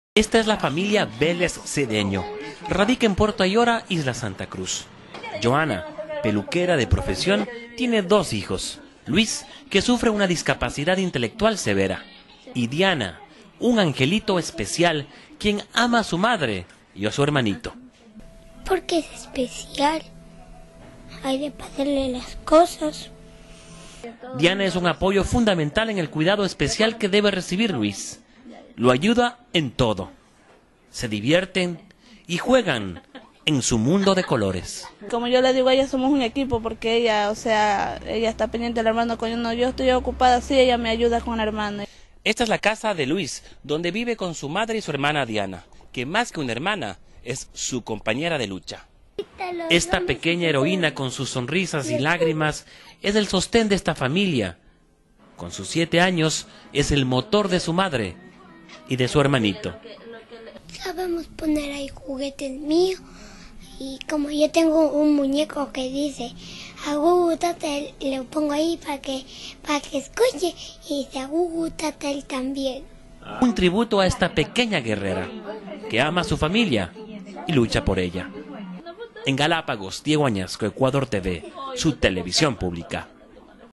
ECTV NOTICIAS -HISTORIA DE UNA NIÑA HEROÍNA DE GALÁPAGOS
Desde Galápagos les presentamos la historia de una heroína de siete años, ella es el soporte de su madre y su hermano con discapacidad  Veamos